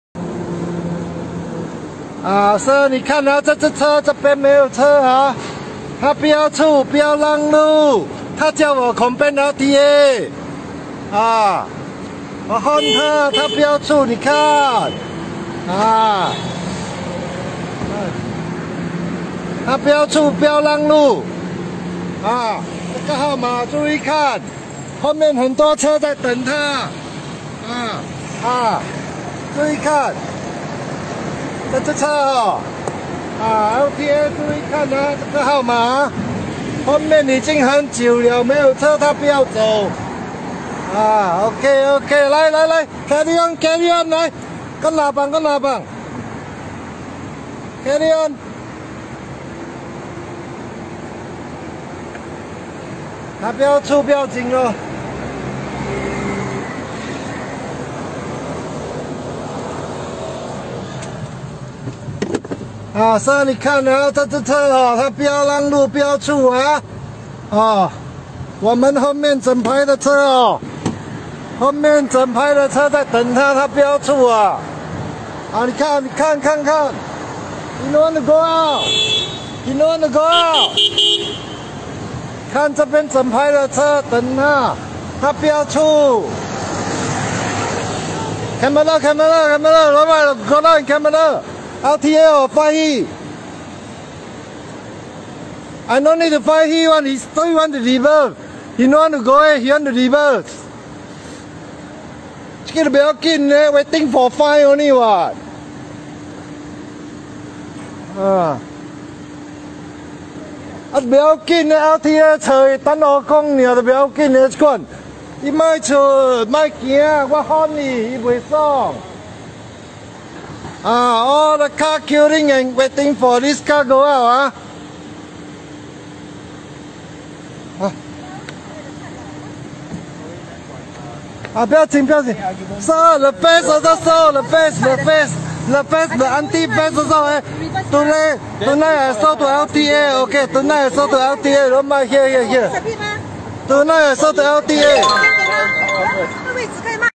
7aug2020 Katong Shopping Centre SMT1249J honda shuttle driver expect everyone to reverse for her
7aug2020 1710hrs
Katong Shopping Centre
SMT1249J honda shuttle
self entitled driver expect everyone to reverse for her to park.
blocking & holding up the driveway